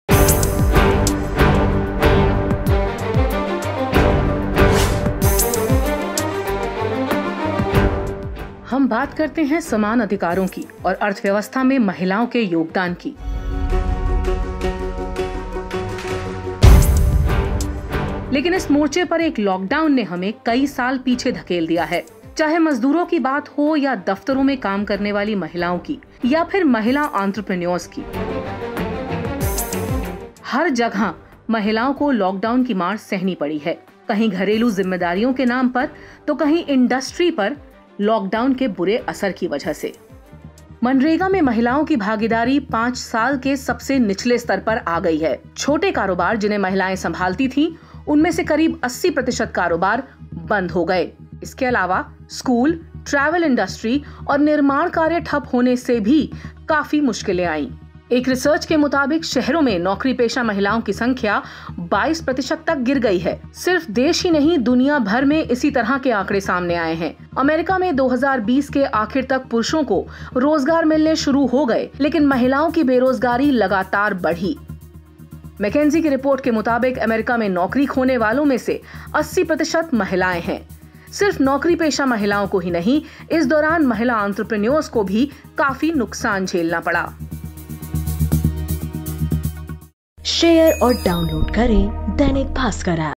महिलाएं लॉक, जॉब डाउन
लॉकडाउन कल, आज और कल: महिलाओं की आपबीती; सैलरी बगैर काम करने को तैयार थीं, तब भी कंपनी ने निकाला